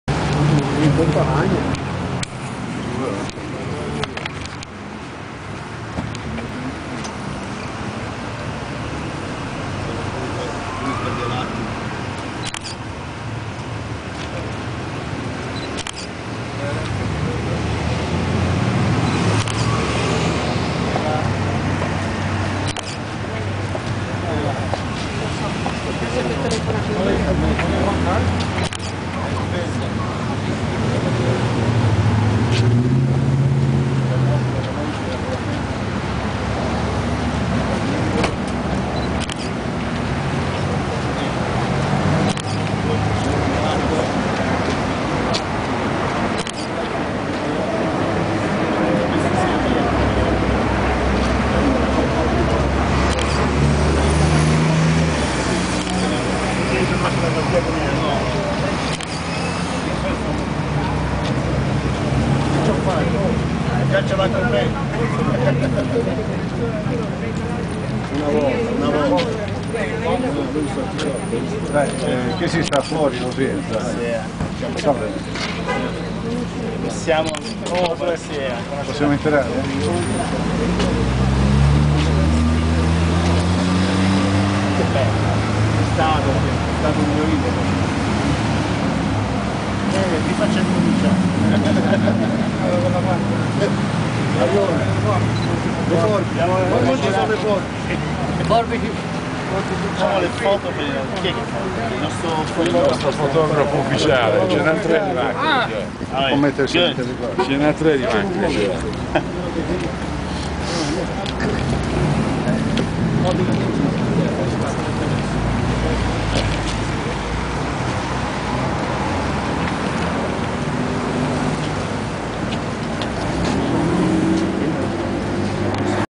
On.Tremaglia inaugura la sede del Comites di Chicago/Sgro`
SEDE